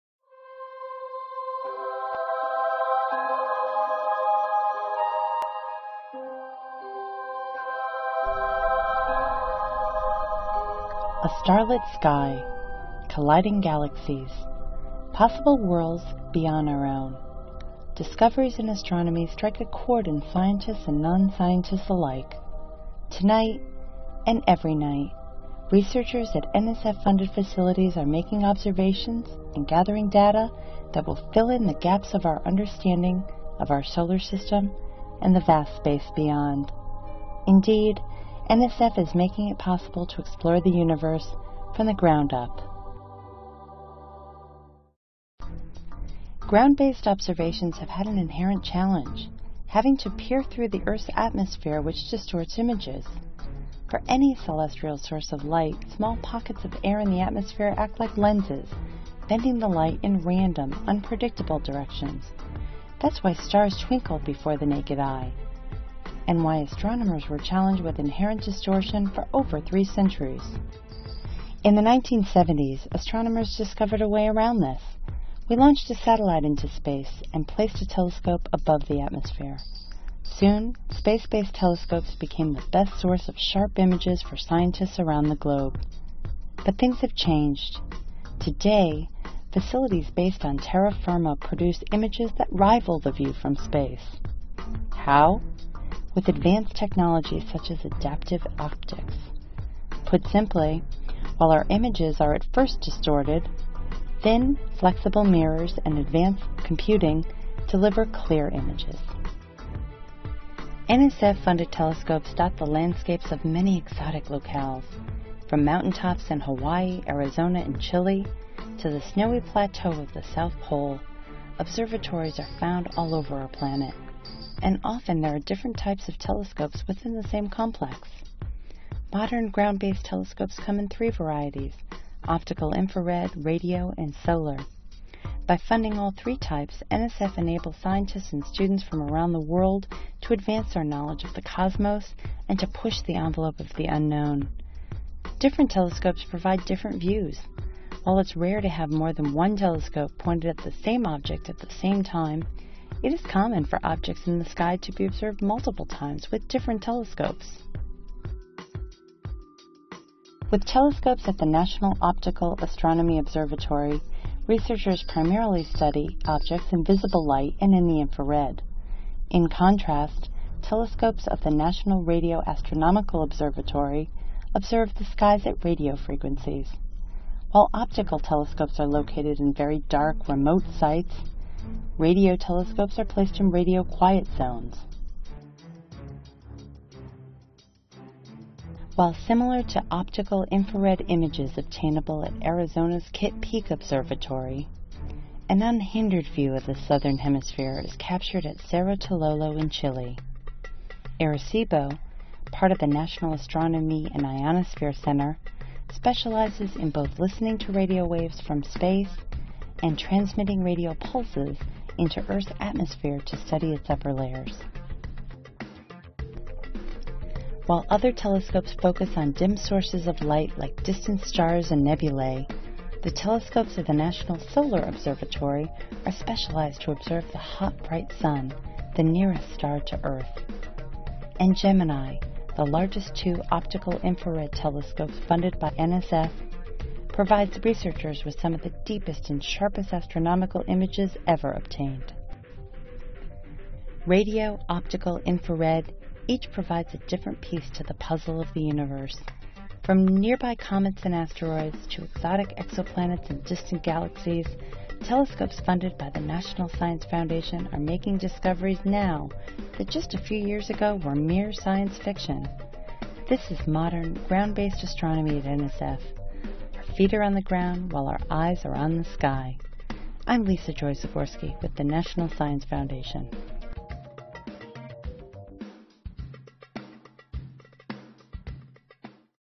From the Ground Up - Eyes on the Sky
NSF is making it possible to explore the universe…from the ground up. Today, NSF funds world-class facilities that use cutting-edge technology, providing images that rival the view from space. This video slideshow gives the viewer a glimpse of some of the tremendous radio, optical, infrared and solar observatories.